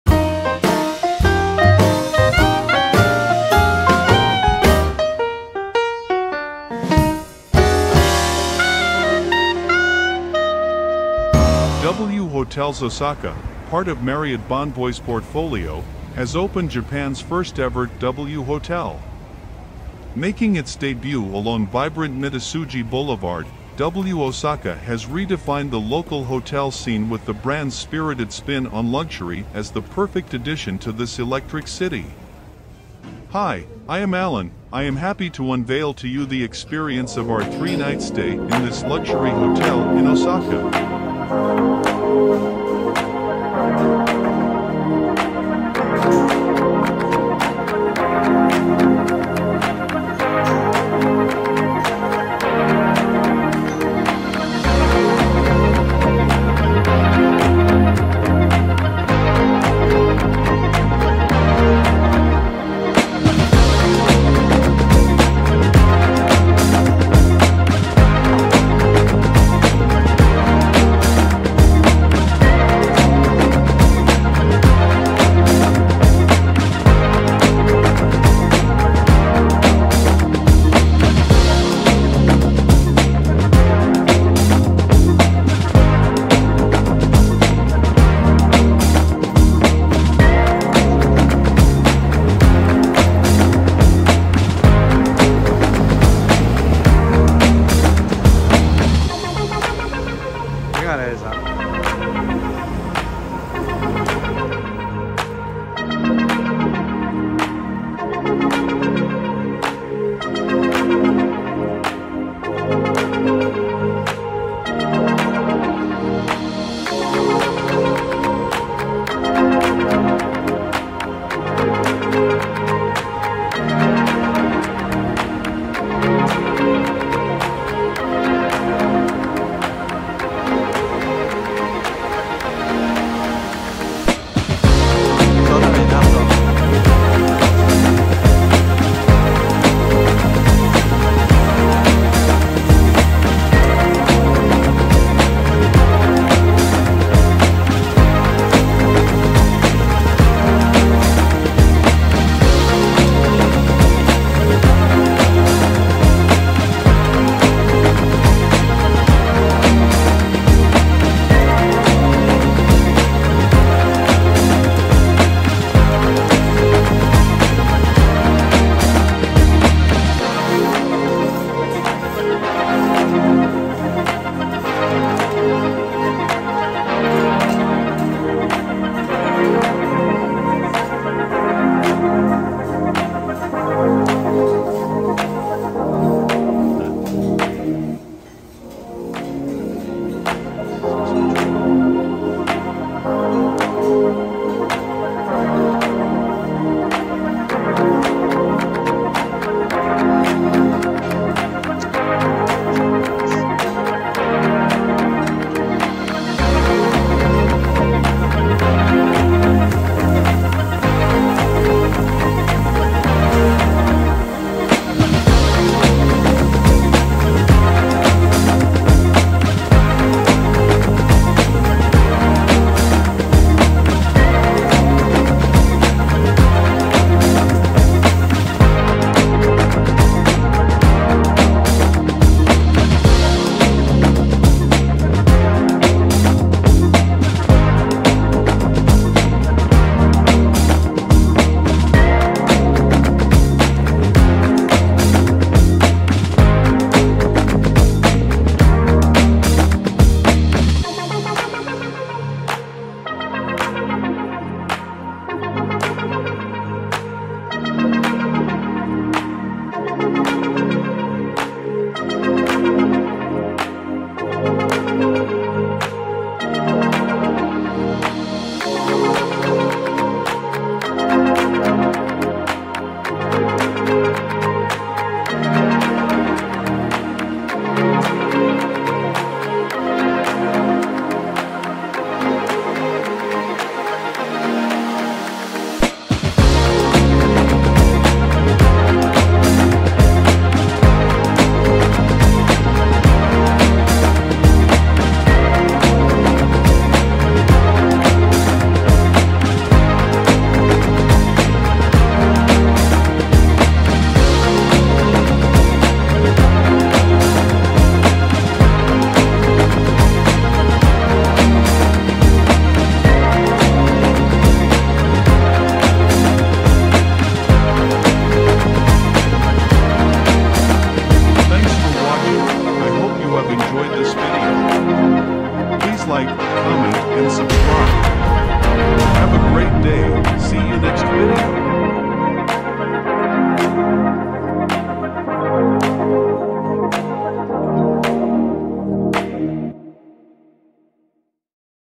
【W Hotel Osaka | 大阪🇯🇵】We spent 4 days 3 nights at the new
@LifeLivesWell Travel to Osaka and tried out the new @WHotelsWorldwide in osaka @osakatravelguide2100 and had @teppanyakisteakmaster , kaiseki meals, shopping and taking the advantage of low @JapaneseCurrency exchanges